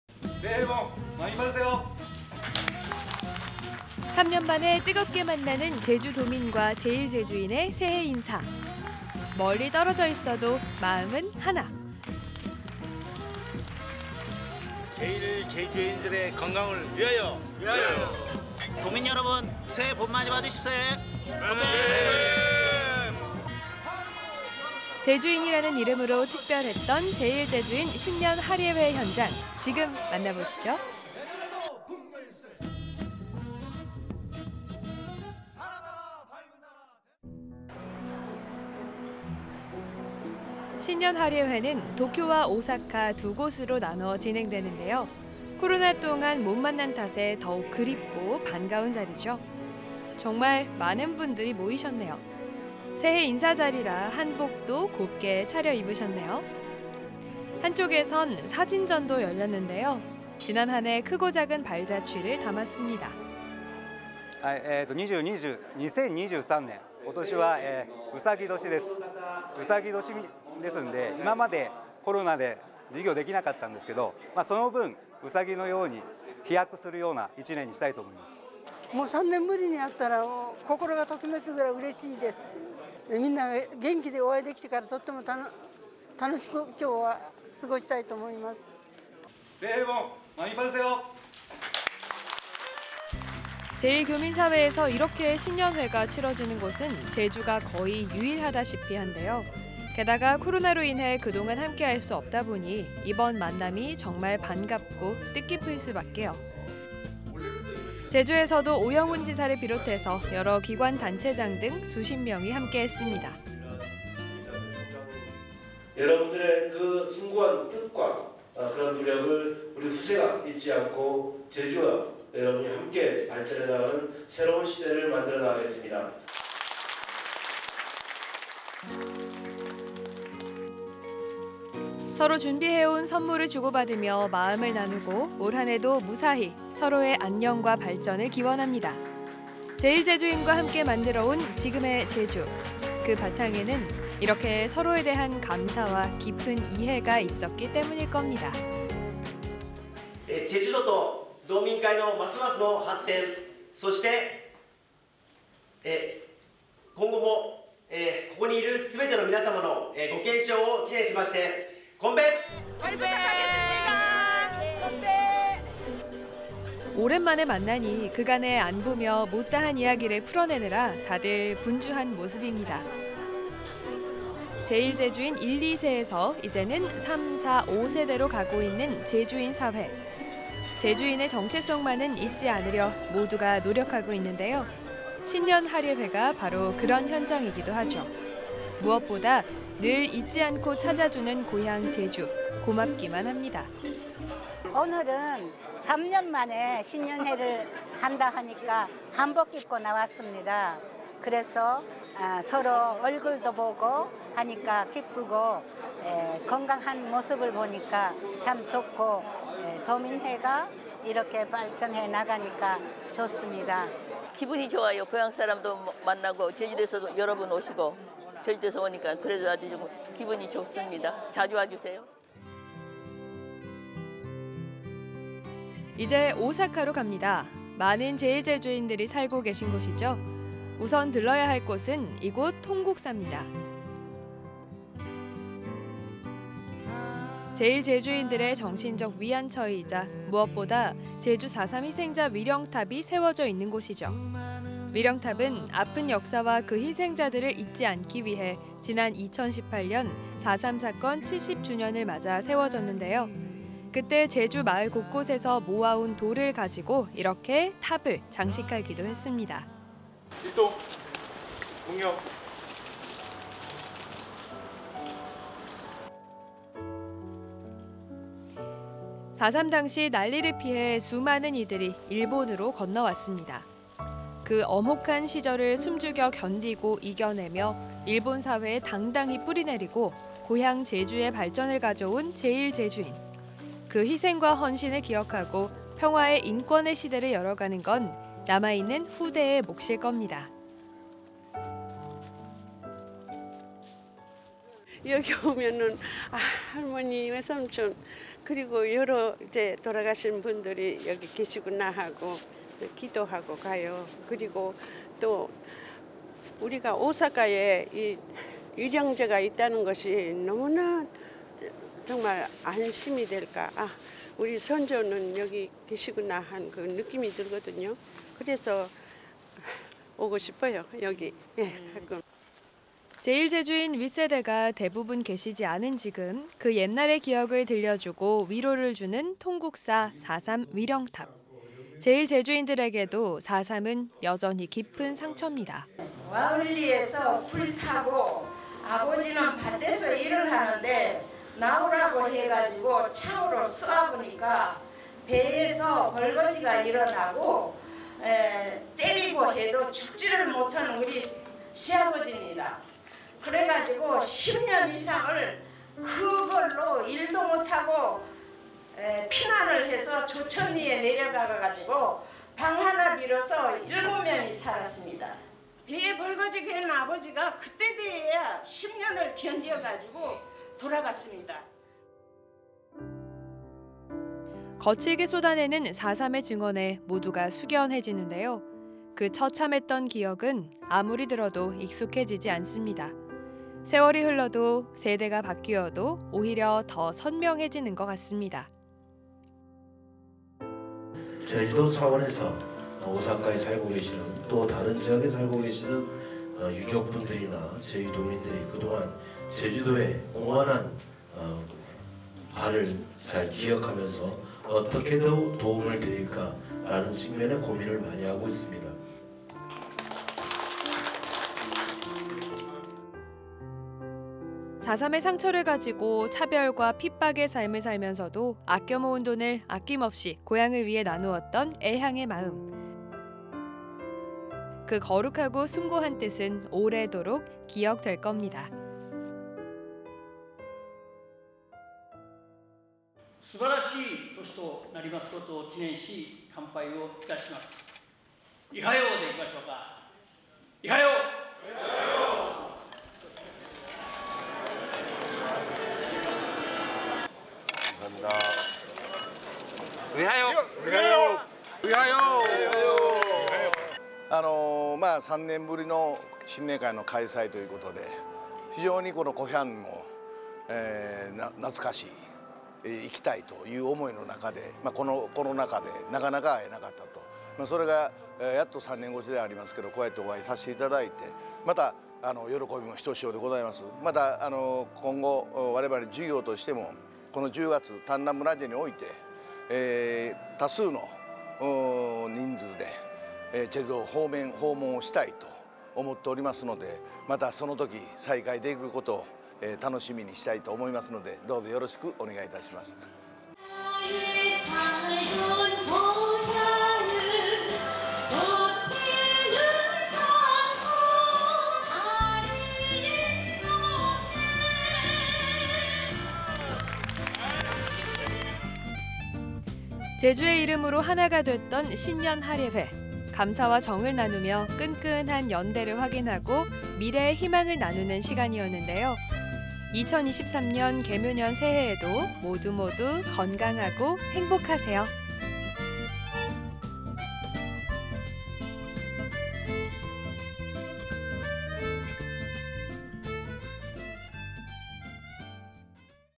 [타임라인] 2023년 재일제주인 신년하례회
코로나 펜데믹으로 인해 3년만에 일본에서 열린
재일제주인들의 신년하례회
우리 동포들의 반가운 얼굴을 만나는 시간